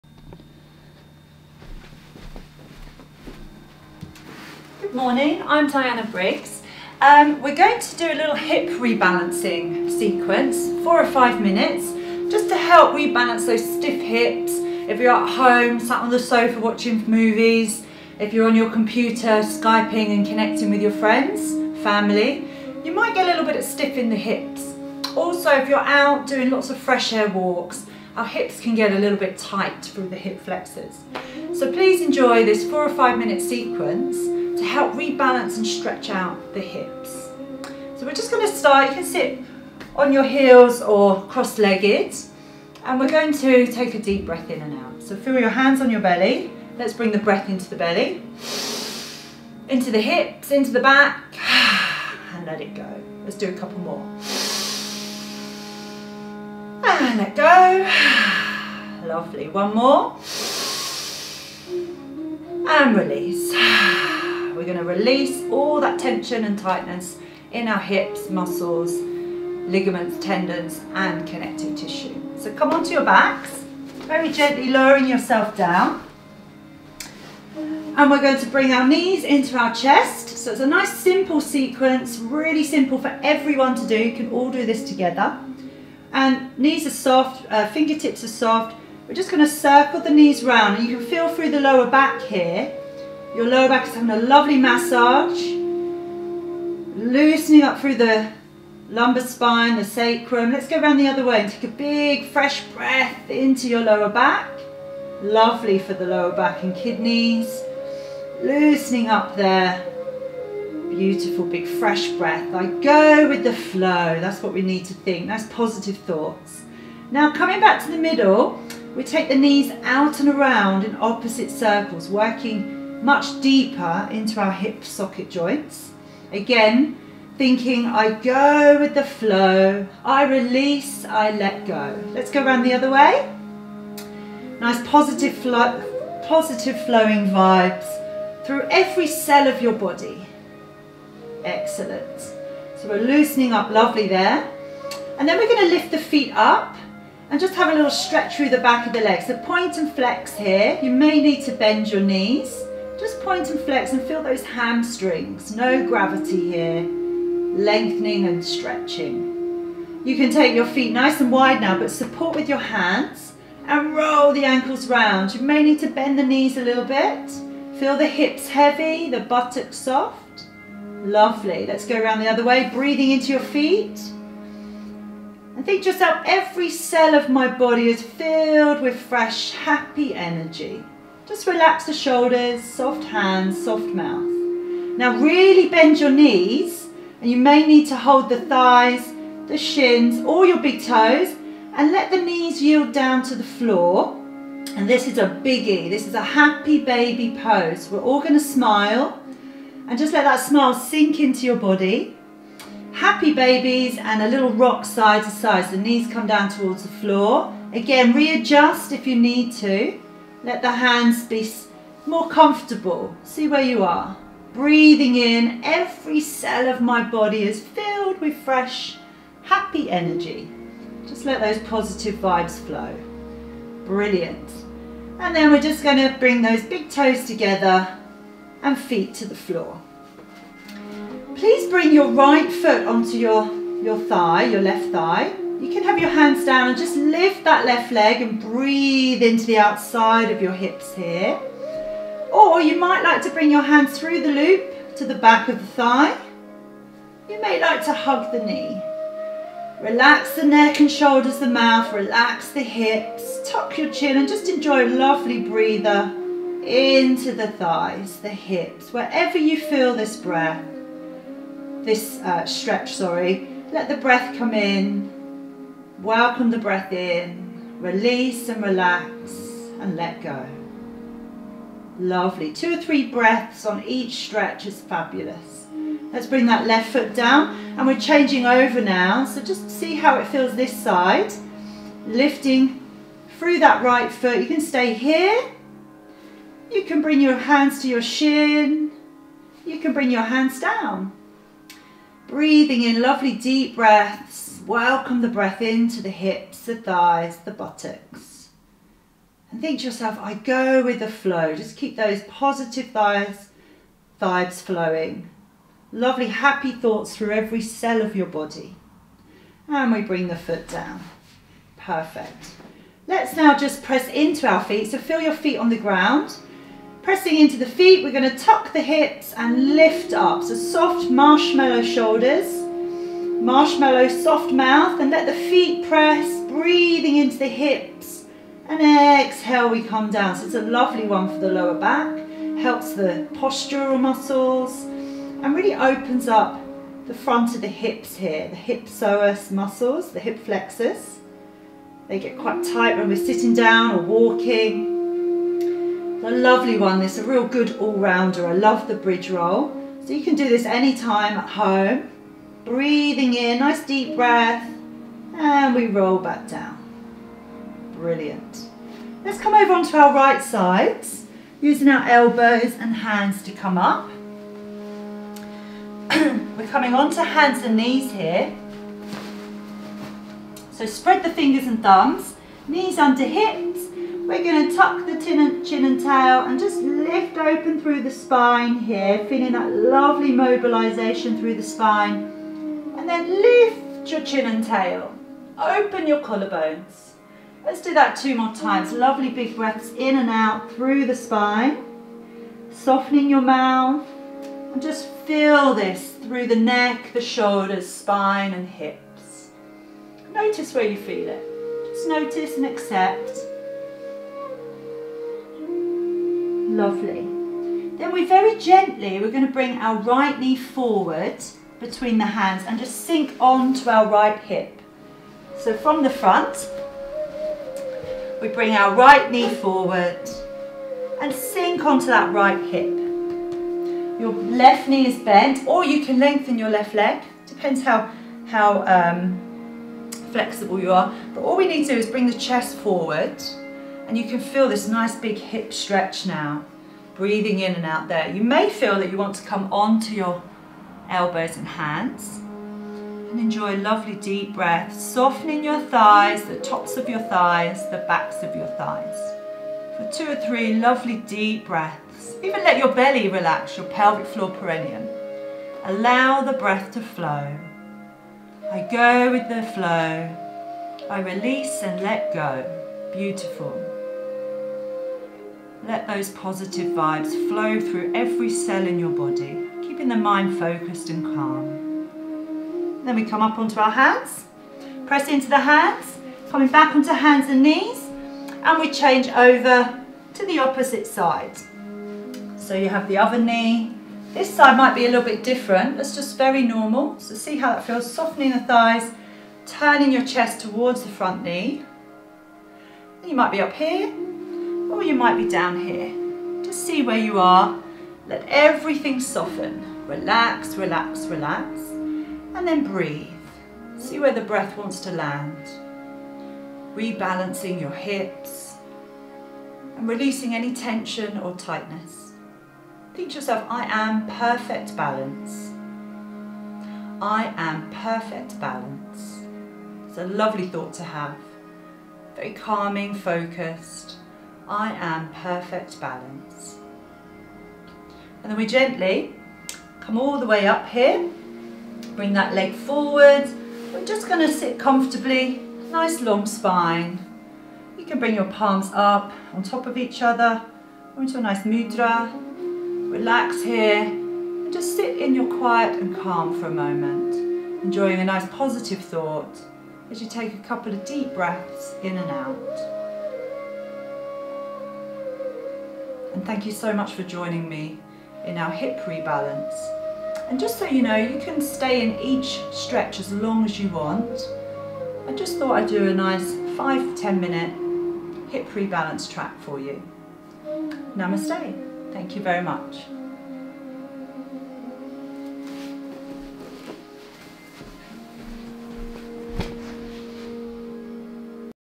Hip Rebalancing Sequence
Experienced Yoga instructor, Diana Briggs, helps rebalance stiff hips - ideal if you've been static for long periods, sitting on the sofa or at a desk for example!